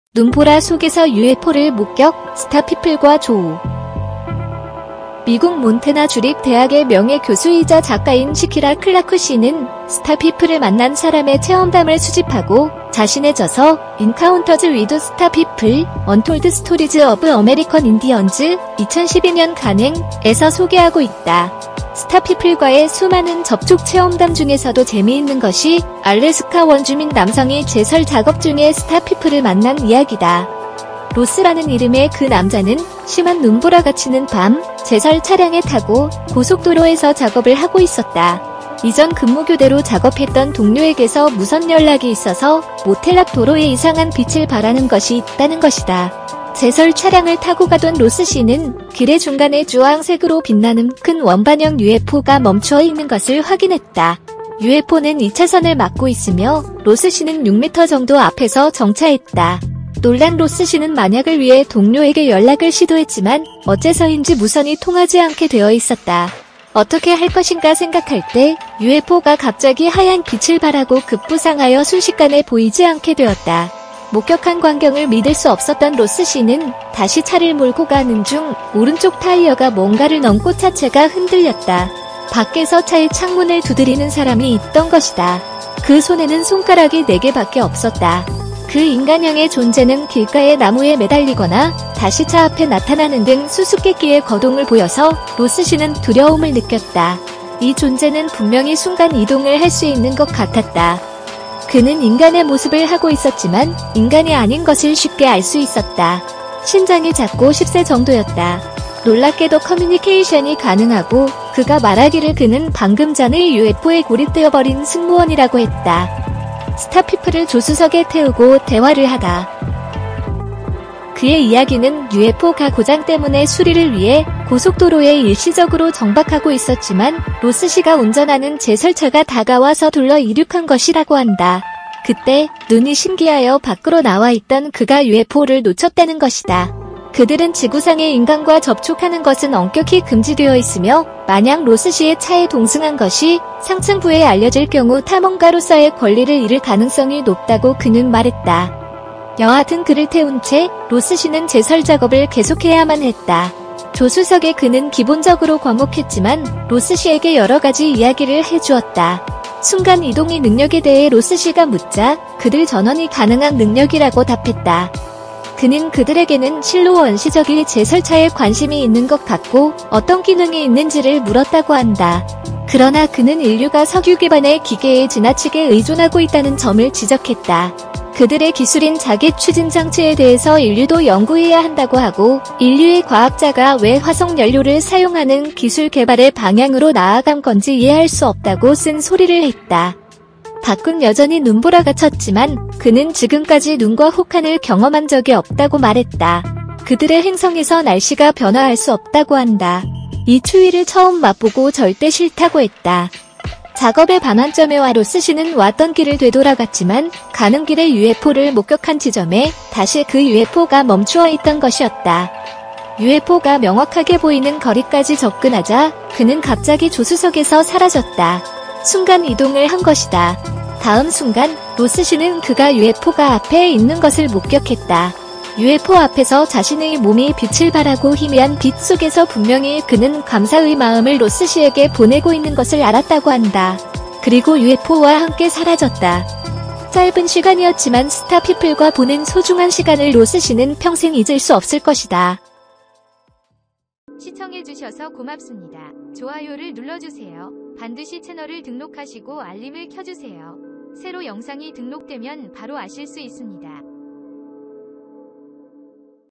스타 피플과의 만남! 손가락 4개, 순간 이동, 인류에 경고... "스타 피플을 차에 태운"남자의 실화!
미국 몬태나 주립 대학의 명예 교수이자 작가인 시키라 클라크 씨는 "스타 피플"을 만난 사람의 체험담을 수집하고